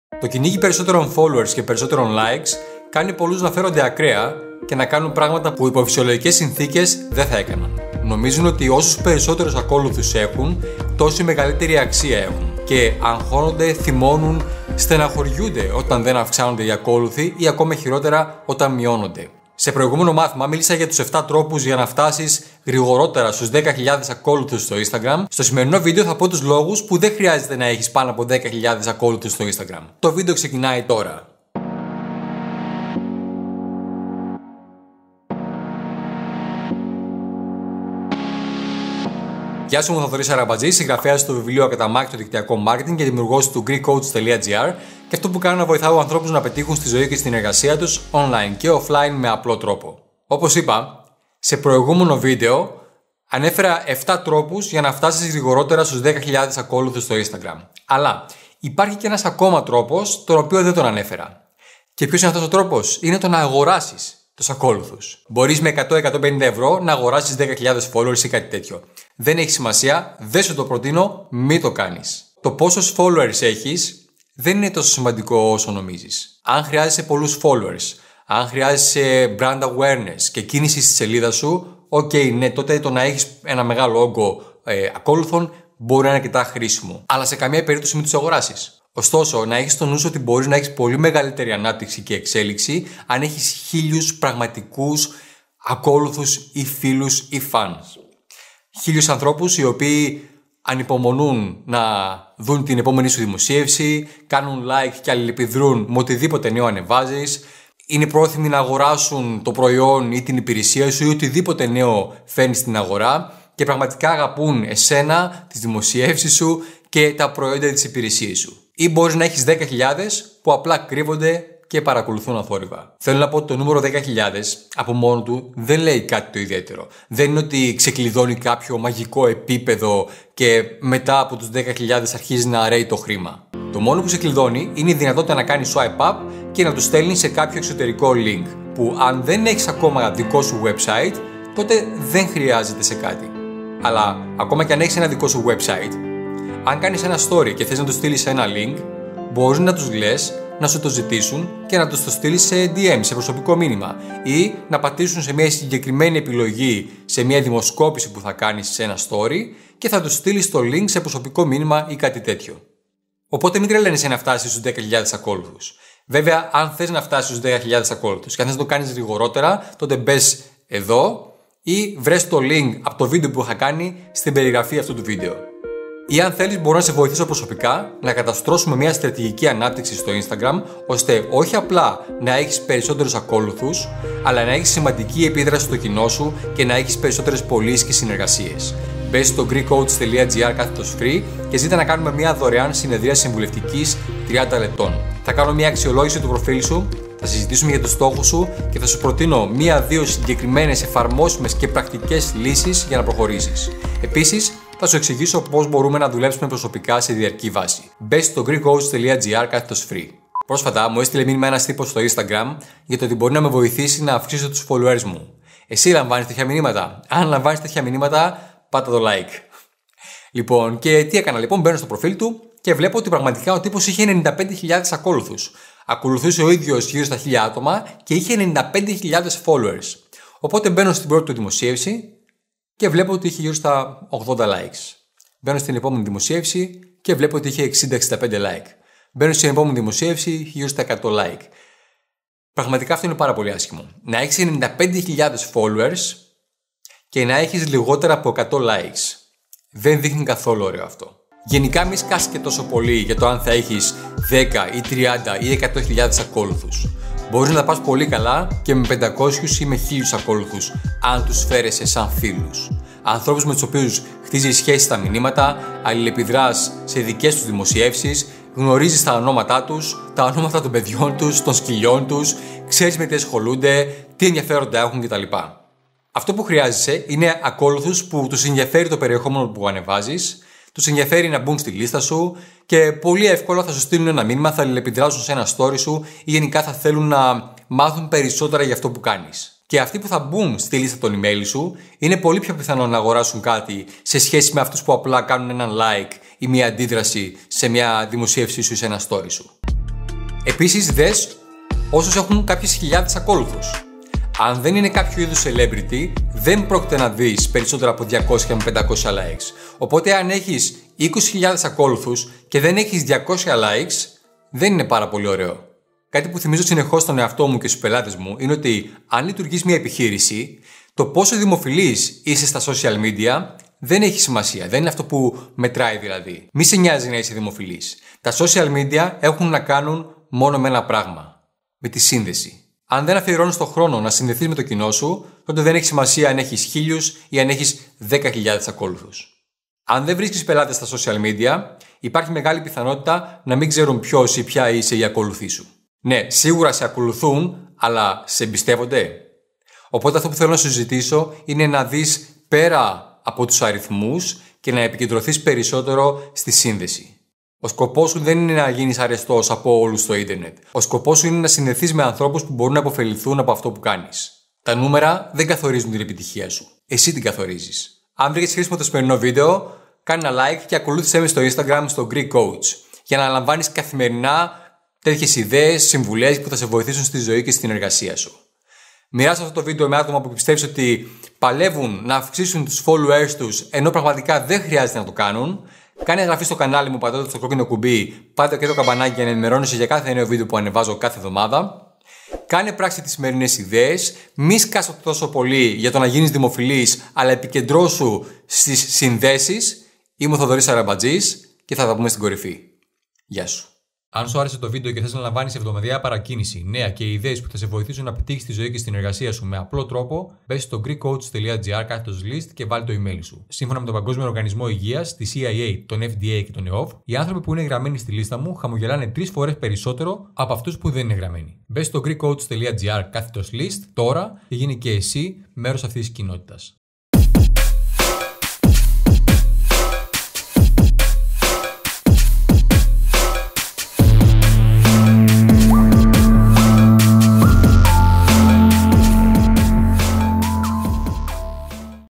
Γιατί ΔΕΝ Χρειάζεσαι 10k Ακόλουθους στο Instagram
Γιατί ΔΕΝ Χρειάζεσαι 10k Ακόλουθους στο Instagram // Χρειάζεσαι άτομα που ΕΝΔΙΑΦΕΡΟΝΤΑΙ για εσένα και το περιεχόμενό σου, όχι απλώς ΠΟΛΛΑ άτομα.

Αν θέλεις να βάλουμε μαζί μία στρατηγική για τα social media, μπες στο και ζήτα να κάνουμε μία δωρεάν συνεδρία συμβουλευτικής.

0:50 ΜΗΝ αγοράσεις followers
1:28 Έχε 1.000 πραγματικούς ακόλουθους και όχι 10.000 ψεύτικους
2:02 Αν θέλεις τους 10k για το swipe up, δες τις εναλλακτικές
2:57 Μπορώ να σε βοηθήσω να βελτιώσεις το προφίλ σου
3:30 Χρειάζεσαι engagement και όχι followers
5:28 Τα social media έχουν να κάνουν με τη ΣΥΝΔΕΣΗ